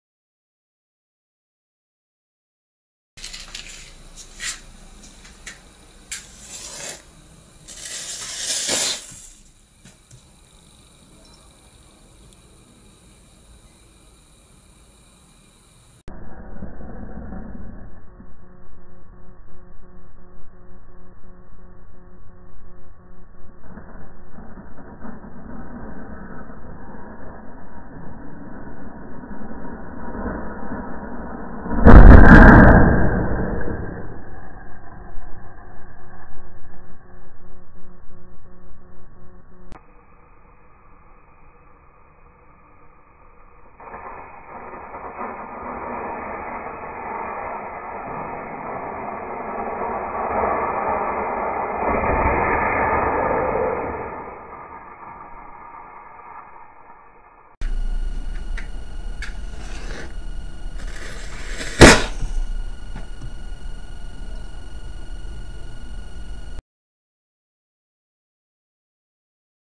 Как стреляют Пушки из винных пробок.
Самодельная пушка своими руками.  Огненный выстрел из самодельной пробковой пушки ядром из фольги.

Пробковые пушки